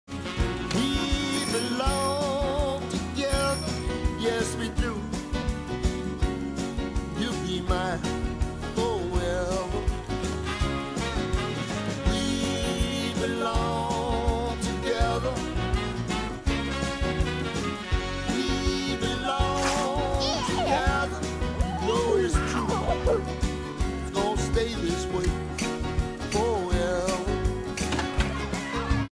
Children story 3 clip 1
Charlie as Ken
Rosie as Barbie
Jack Jack as Big baby
Curious George has Cymbal banging monkey